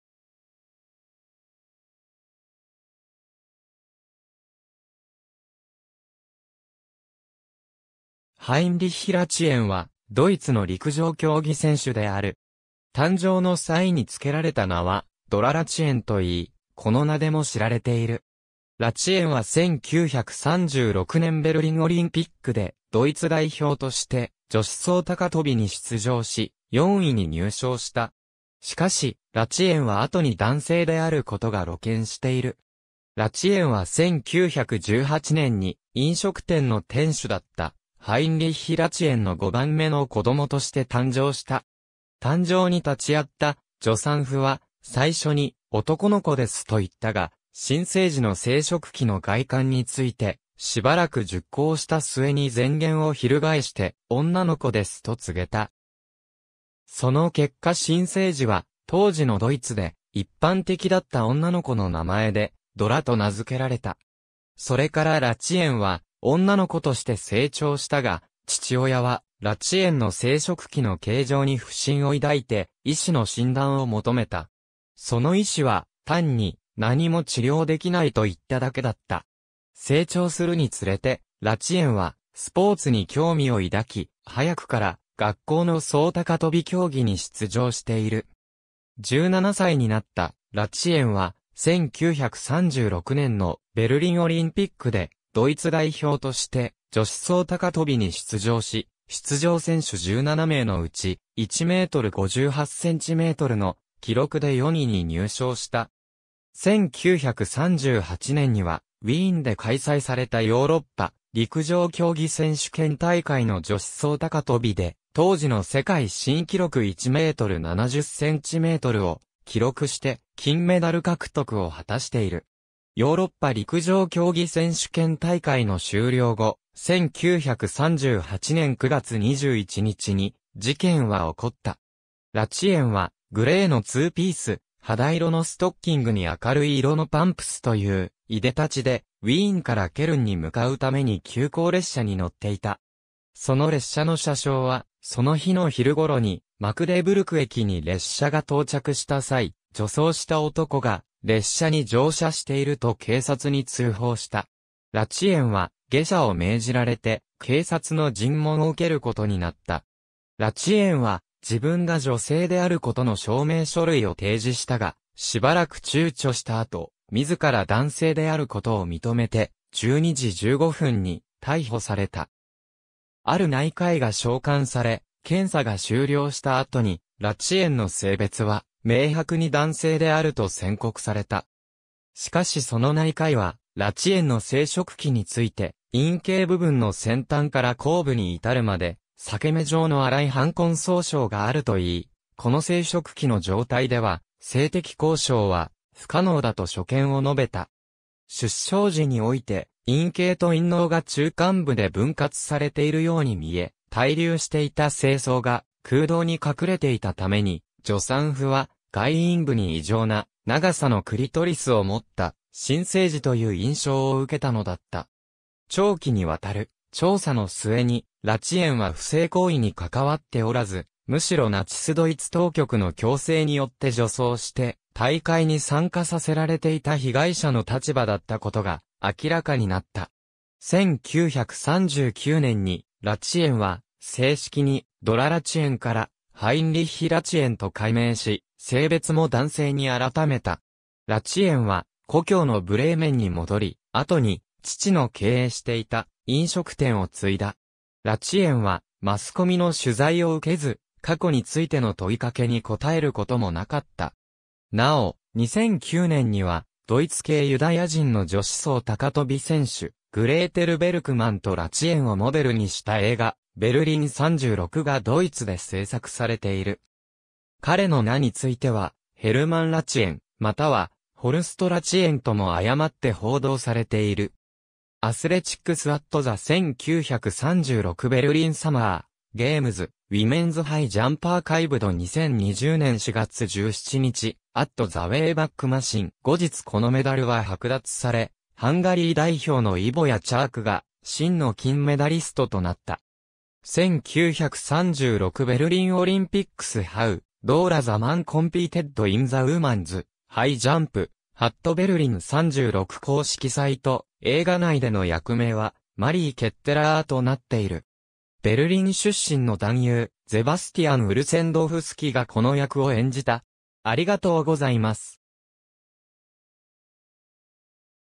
ドラ・ラチエン
ハインリッヒ・ラチエン（Heinrich Ratjen、1918年11月20日 – 2008年4月22日）は、ドイツの陸上競技選手である。
誕生の際につけられた名は、ドラ・ラチエン（Dora Ratjen）といい、この名でも知られている。
ラチエンは1936年ベルリンオリンピックでドイツ代表として女子走高跳に出場し、4位に入賞した。
しかし、ラチエンは後に男性であることが露顕している。
ラチエンは1918年に、飲食店の店主だったハインリッヒ・ラチエンの5番目の子供として誕生した。
誕生に立ち会った助産婦は、最初に「男の子です」と言ったが、新生児の生殖器の外観についてしばらく熟考した末に前言を翻して「女の子です」と告げた。
その結果新生児は、当時のドイツで一般的だった女の子の名前で「ドラ」と名づけられた。
それからラチエンは女の子として成長したが、父親はラチエンの生殖器の形状に不審を抱いて医師の診断を求めた。
その医師は単に「何も治療できない」と言っただけだった。
成長するにつれて、ラチエンはスポーツに興味を抱き、早くから学校の走高跳競技に出場している。
17歳になったラチエンは、1936年のベルリンオリンピックでドイツ代表として女子走高跳に出場し、出場選手17名のうち、1m58cmの記録で4位に入賞した。
1938年には、ウィーンで開催されたヨーロッパ陸上競技選手権大会の女子走高跳で当時の世界新記録1m70cmを記録して金メダル獲得を果たしている。
ヨーロッパ陸上競技選手権大会の終了後、1938年9月21日に事件は起こった。
ラチエンはグレーのツーピース、肌色のストッキングに明るい色のパンプスといういでたちで、ウィーンからケルンに向かうために急行列車に乗っていた。
その列車の車掌は、その日の昼頃にマクデブルク駅に列車が到着した際、「女装した男が列車に乗車している」と警察に通報した。
ラチエンは下車を命じられて、警察の尋問を受けることになった。
ラチエンは自分が女性であることの証明書類を提示したが、しばらく躊躇した後、自ら男性であることを認めて、12時15分に逮捕された。
ある内科医が召喚され、検査が終了した後にラチエンの性別は明白に男性であると宣告された。
しかしその内科医は、ラチエンの生殖器について「陰茎部分の先端から後部に至るまで裂目状の粗い瘢痕創傷」があると言い、この生殖器の状態では性的交渉は不可能だと所見を述べた。
出生時において、陰茎と陰嚢が中間部で分割されているように見え、滞留していた精巣が空洞に隠れていたために、助産婦は外陰部に異常な長さのクリトリスを持った新生児という印象を受けたのだった。
長期にわたる調査の末に、ラチエンは不正行為にかかわっておらず、むしろナチス・ドイツ当局の強制によって女装して大会に参加させられていた被害者の立場だったことが明らかになった。
1939年に、ラチエンは正式に「ドラ・ラチエン」から「ハインリッヒ・ラチエン」と改名し、性別も男性に改めた。
ラチエンは故郷のブレーメンに戻り、後に父の経営していた飲食店を継いだ。
ラチエンはマスコミの取材を受けず、過去についての問いかけに答えることもなかった。
なお、2009年にはドイツ系ユダヤ人の女子走高跳選手グレーテル・ベルクマンとラチエンをモデルにした映画『Berlin 36』 (Berlin 36) がドイツで製作されている。
^ 彼の名については、ヘルマン・ラチエン（Hermann Ratjen）、またはホルスト・ラチエン（Horst Ratjen）とも誤って報道されている。
^ Athletics at the 1936 Berlin Summer Games:Women's High JumpArchived 2020年4月17日, at the Wayback Machine.（英語） ^ 後日このメダルは剥奪され、ハンガリー代表のイボヤ・チャークが真の金メダリストとなった。
^ 1936 Berlin Olympics How Dora the Man Competed in the Woman's High Jump（デア・シュピーゲル on line、2010年5月27日閲覧、英語版） ^ Berlin'36公式サイト（2010年5月27日閲覧、ドイツ語） ^ 映画内での役名は「マリー・ケッテラー（Marie Ketteler...